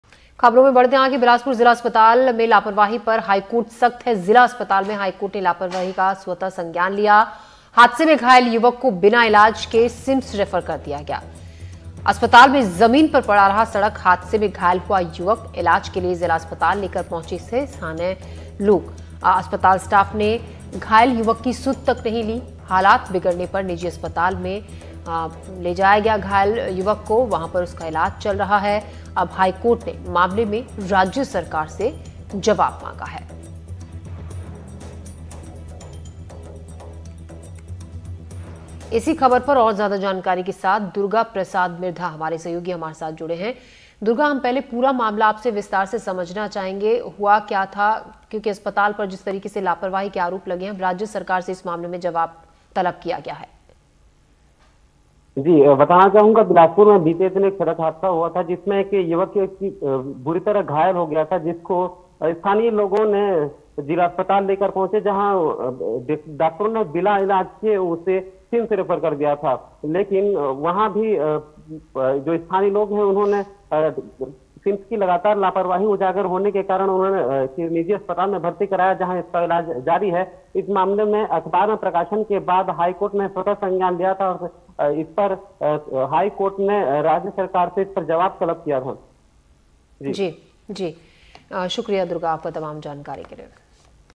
Bilaspur : जिला अस्पताल में लापरवाही पर High Court सख्त | Breaking | Latest News | Chhattisgarh News
बिलासपुर (Bilaspur)  जिला अस्पताल में लापरवाही पर HC सख्त. हादसे में घायल युवक को बिना इलाज किया था सिम्स रेफर (Sims Refer). हॉस्पिटल में जमीन पर पड़ा रहा सड़क हादसे में घायल हुआ युवक. अब हालात बिगड़ने पर निजी अस्पताल में चल रहा इलाज. हाईकोर्ट (High Court) ने मामले में मांगा राज्य सरकार से जवाब